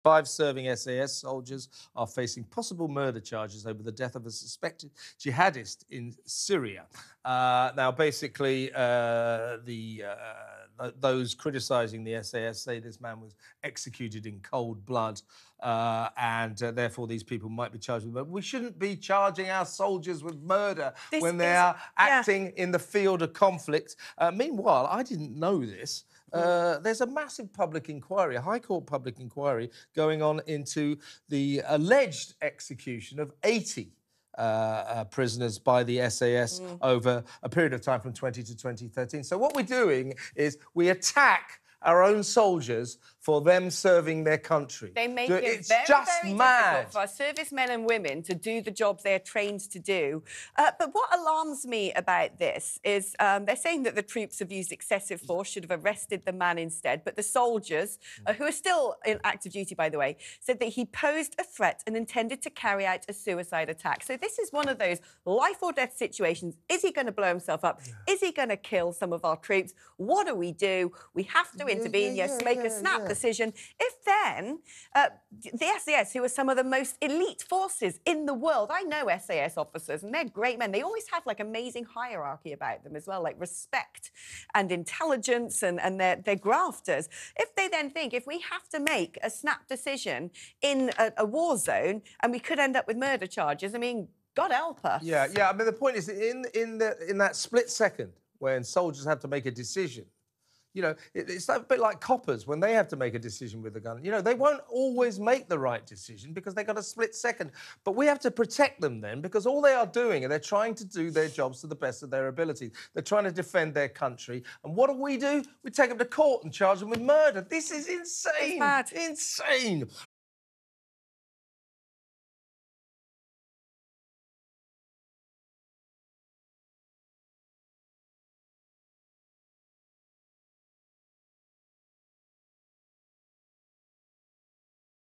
"This is INSANE!" | Five Serving SAS Soldiers Face Murder Charges
Five serving SAS soldiers are facing a murder investigation after the death of a suspected jihadist, according to reports in the Daily Mail.

It's understood that special forces bosses believe troops used excessive force and could have arrested a suspect, who was shot dead in Syria two years ago.

It has been reported that the Royal Military Police are investigating whether the man’s shooting was a war crime.

Soldiers allegedly believed the suspect was wearing a suicide vest and was intent on attacking British troops.